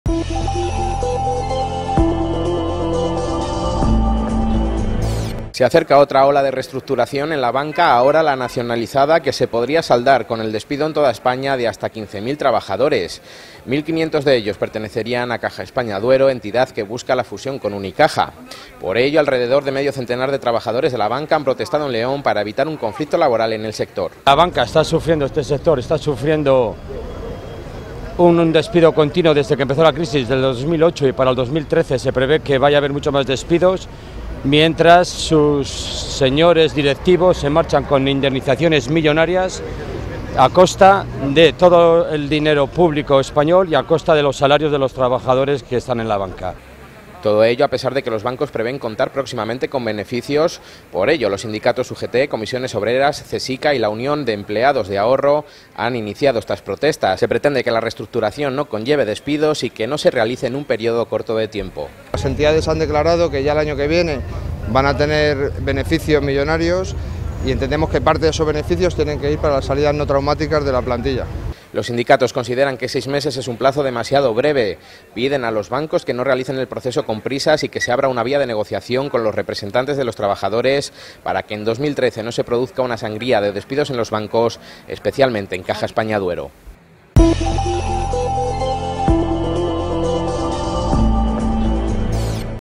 Los sindicatos de banca piden evitar con los beneficios los despidos en el sector
Los sindicatos FES-UGT, Comfia-CCOO, Csica y la Unión de Empleados de Ahorro han pedido este lunes que se deriven los "beneficios millonarios" con los que la banca asegura contar a lo largo del año 2013 para que haya una "salida no traumática" para los empleados del sector ante una nueva "ola de reestructuración" bancaria con los últimos procesos de nacionalización de las entidades